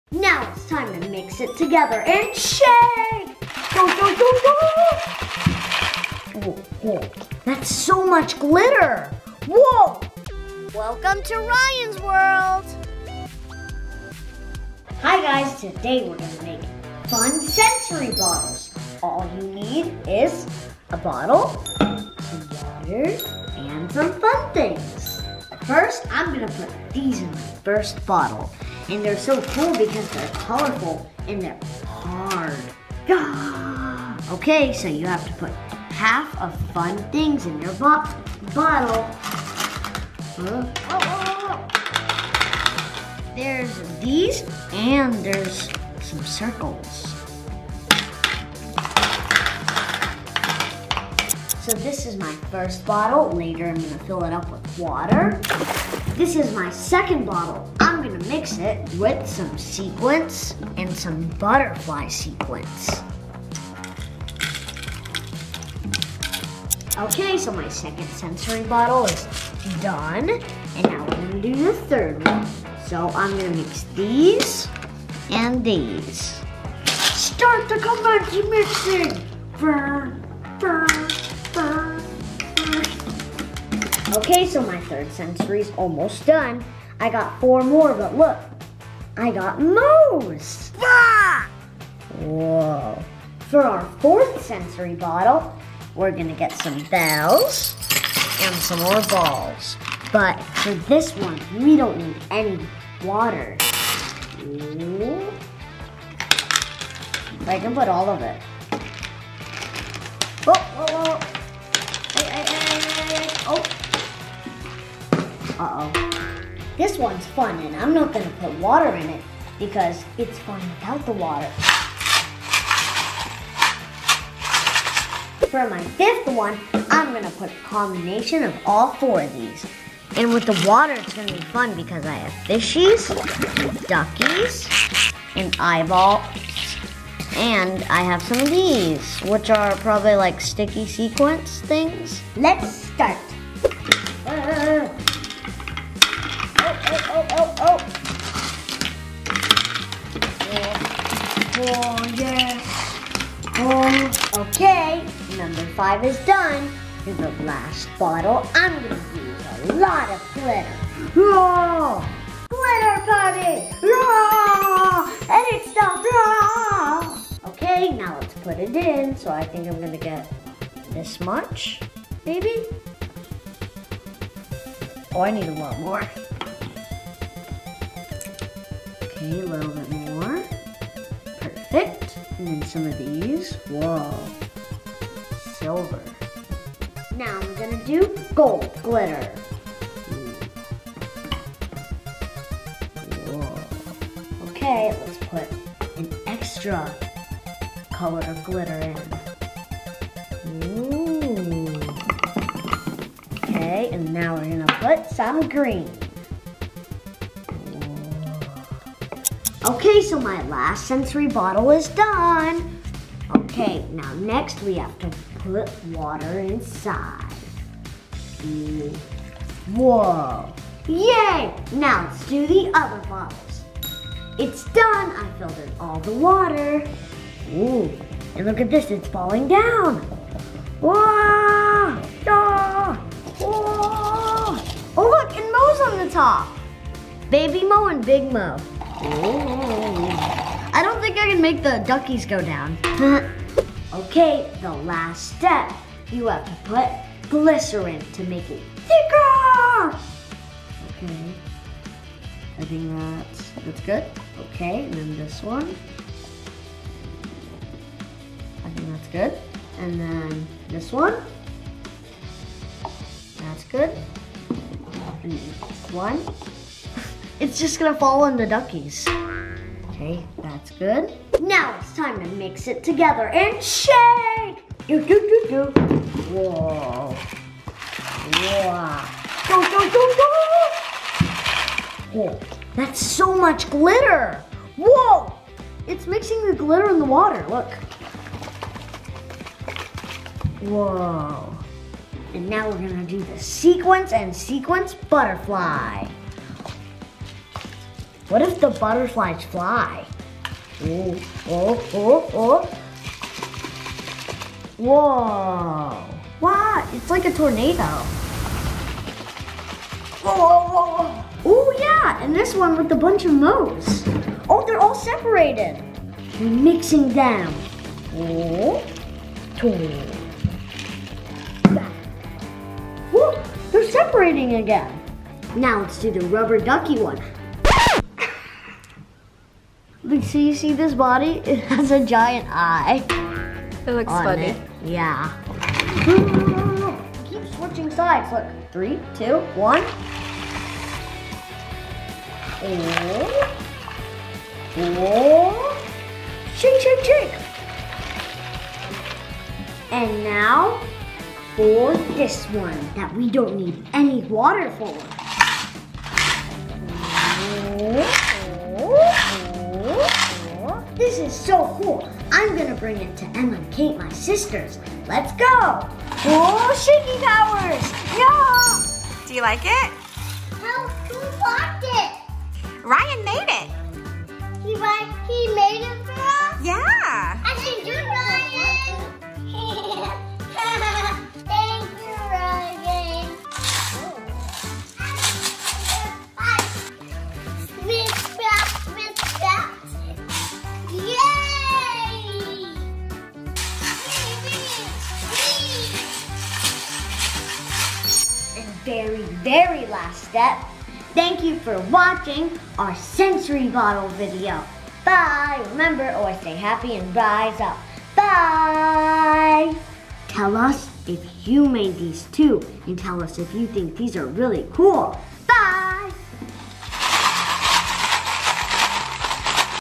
How to make PLAY sensory water bottles for Kids DIY!
How to make PLAY sensory water bottles for Kids! Fun DIY activities for Children with Ryan's World!